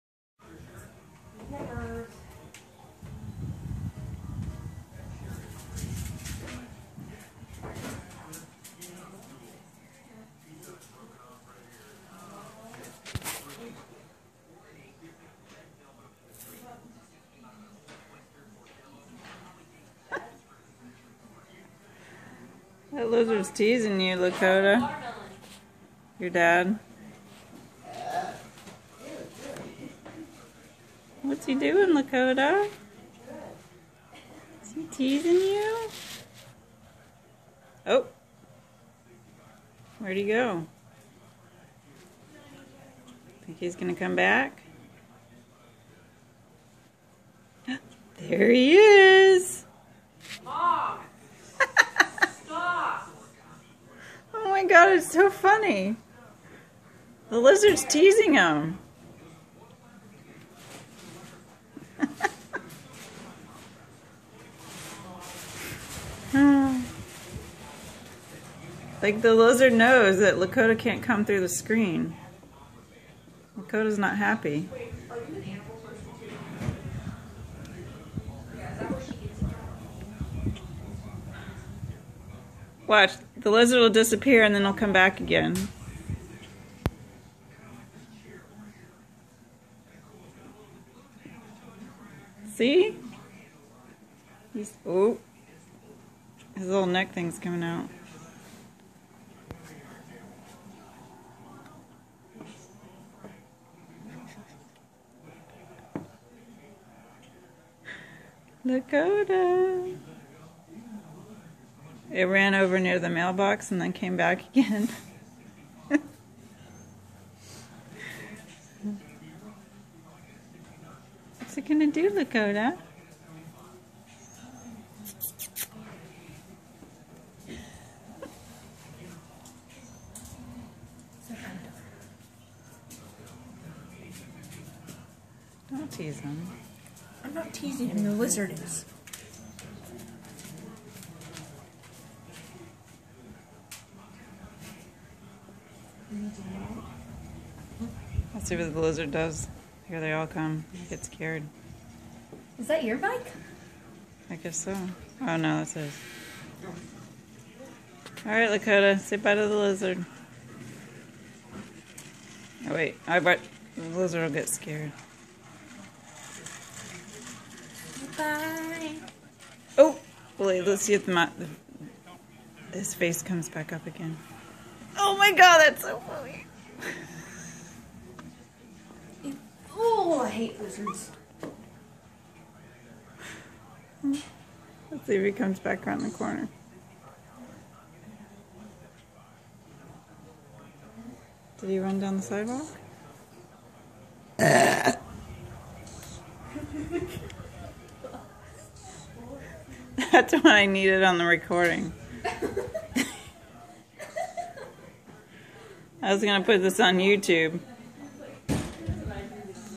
Lizard teasing cat!
A lizard that teases our cat every afternoon. the lizard realizes that out Lakota cannot come through the screen!